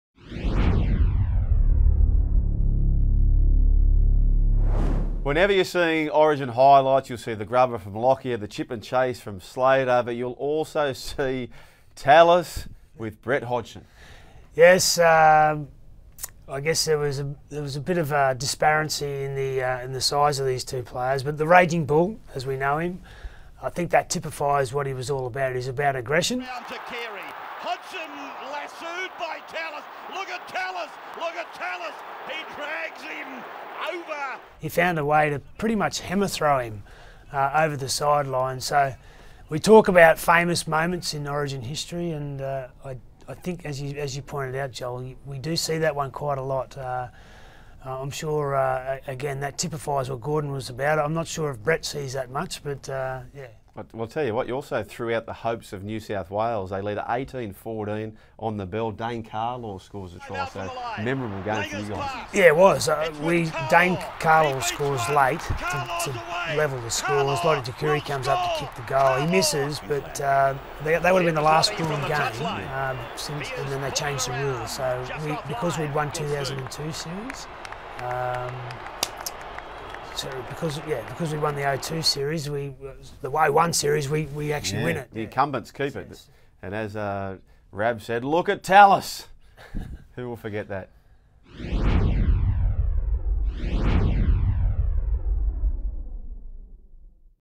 Great Origin Stories: Look at Tallis
Queensland great, Darren Lockyer shares one of his favourite Origin stories.